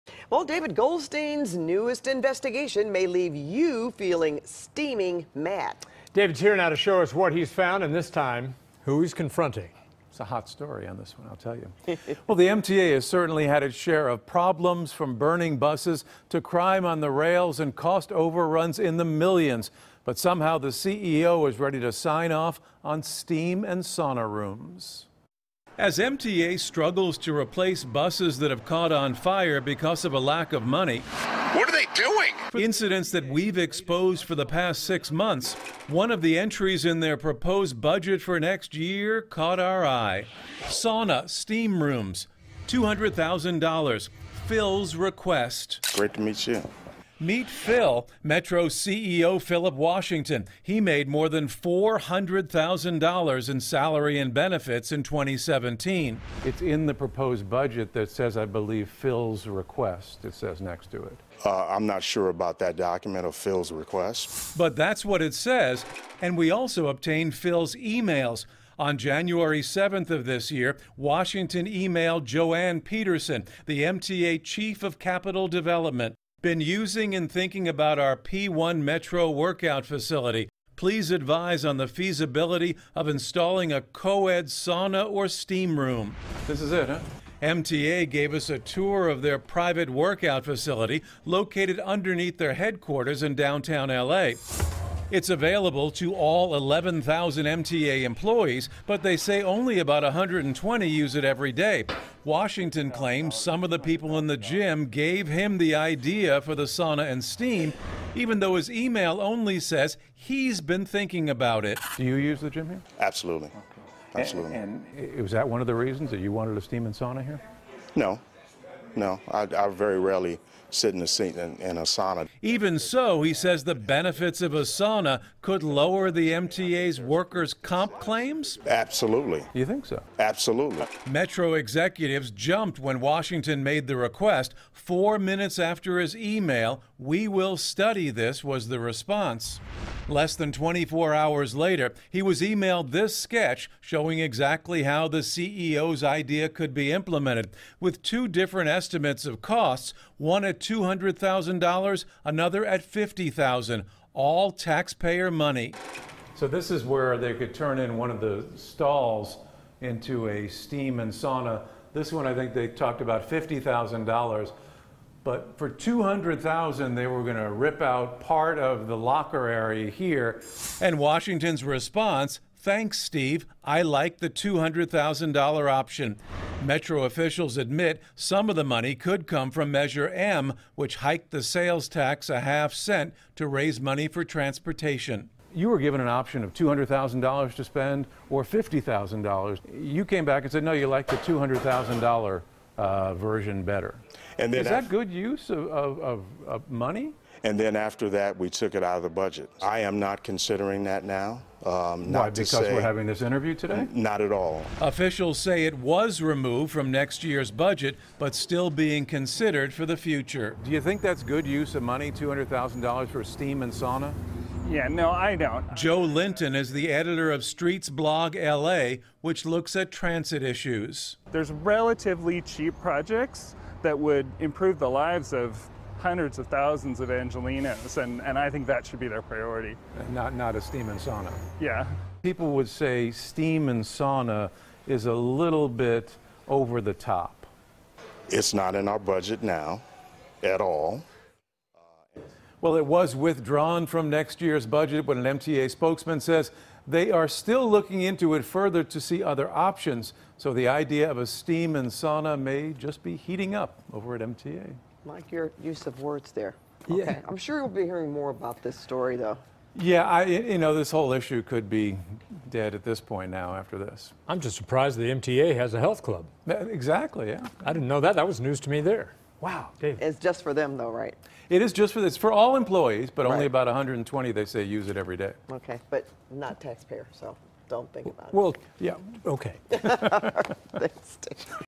Goldstein Investigates: Metro Proposes Plans To Spend $200K On Saunas And Steam Rooms
While the L.A. County Metropolitan Transportation Authority struggles to replace buses that broke down and caught fire because of a lack of money, the agency is proposing spending hundreds of thousands of dollars on steam and sauna rooms in a fitness center used by its employees.